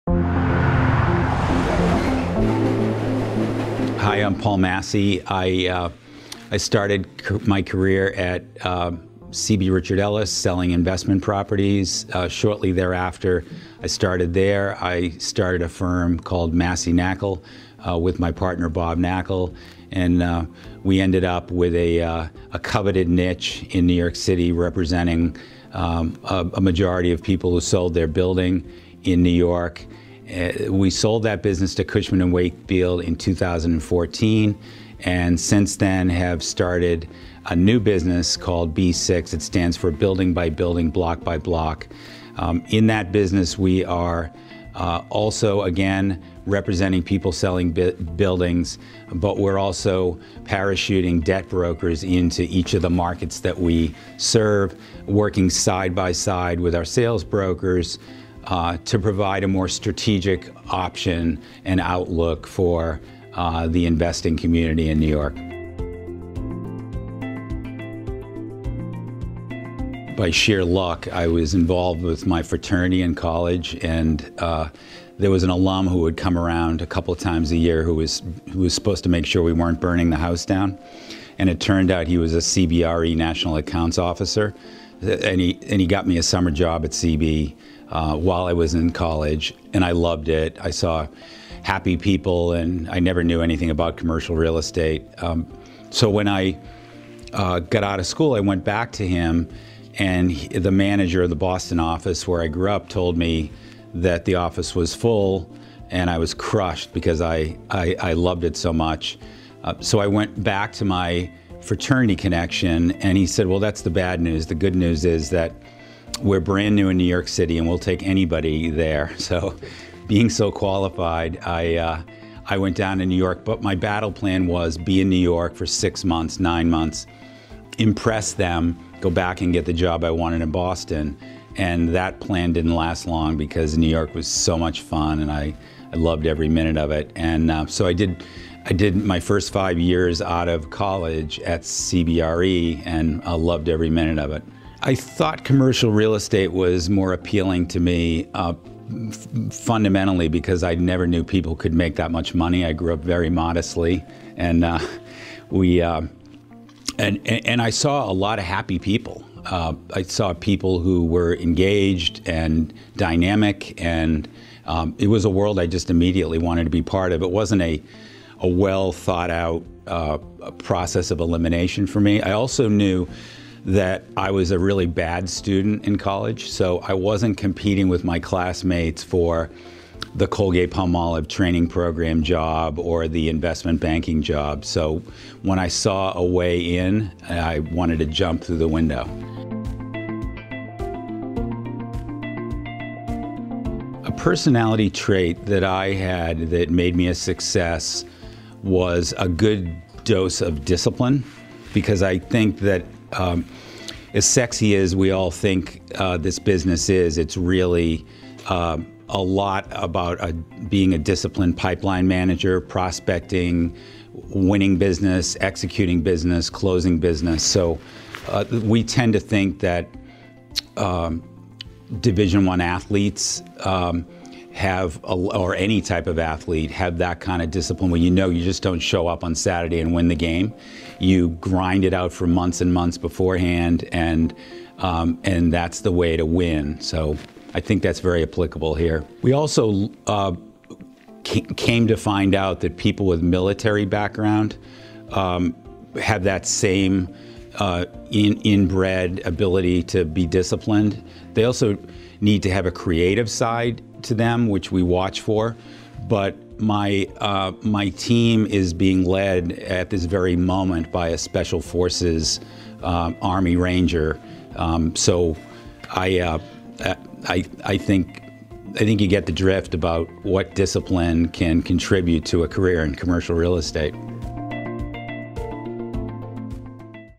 Paul Massey's Real Estate Master Class
Paul Massey founded one of the city's largest investment sales brokerage, Massey Knakal, which sold for $100 million in 2014. The exec, who's since run for mayor and opened a new firm, B6, created and perfected strategies like the territory system, which helped his brokerage dominate the market. How did those systems work, and which ones are coming with him to B6? Find out in this series.